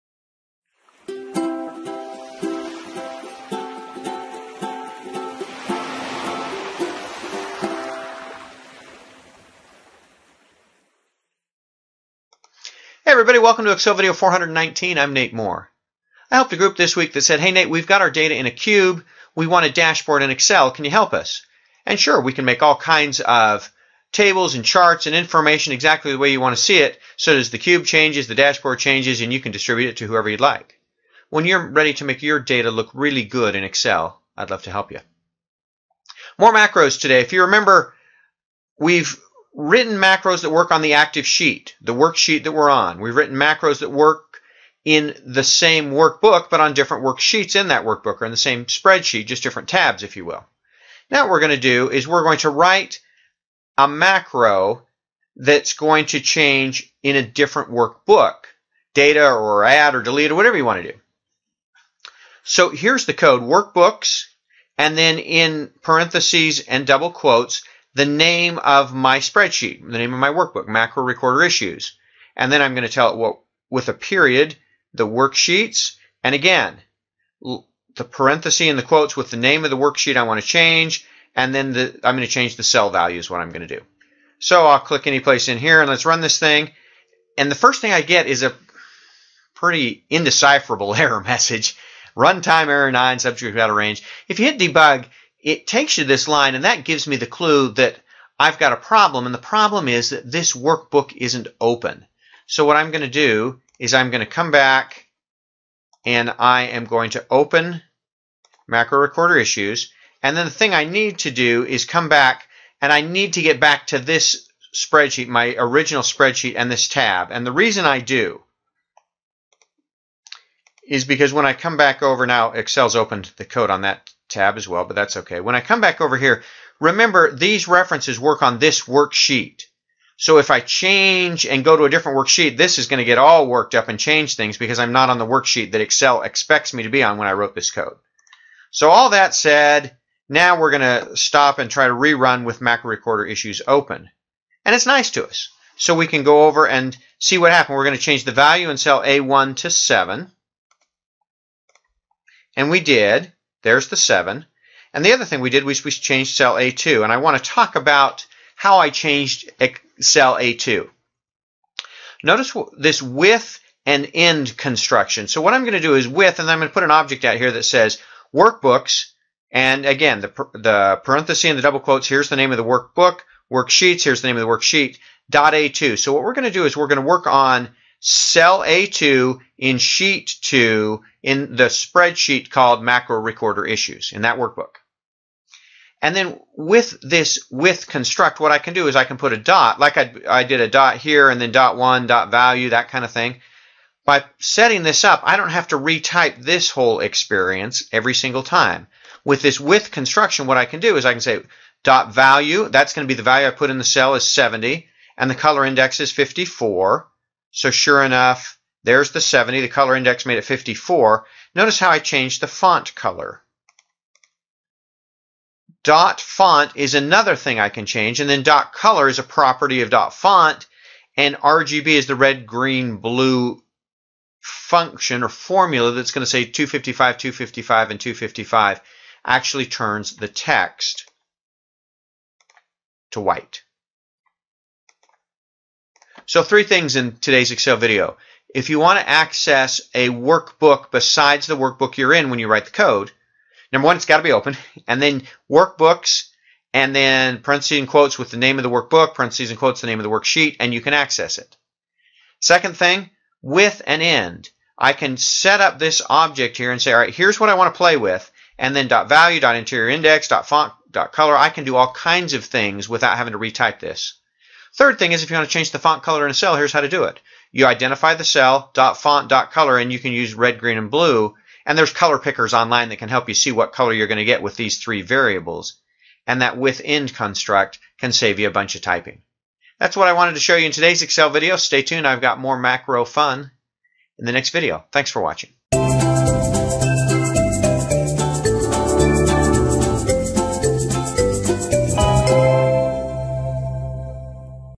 Excel Video 419 Using a Macro to Change Another Workbook
If you’d like to use a macro in one spreadsheet to change another spreadsheet, watch Excel Video 419.  Today we’ll review the syntax to reference another workbook from VBA.  The first thing you’ll notice is that the second workbook has to be open for the VBA code to work.

I’ll also introduce the With…End With syntax.  It’s a great way to reference a cell or a range of cells once and then do several things without having to type in the reference each time.

Finally, I’ll show you how to change the font color in a cell.  If you aren’t familiar with the RGB way of referencing colors, there are several color pickers on the internet that can help you get just the color you’re looking for.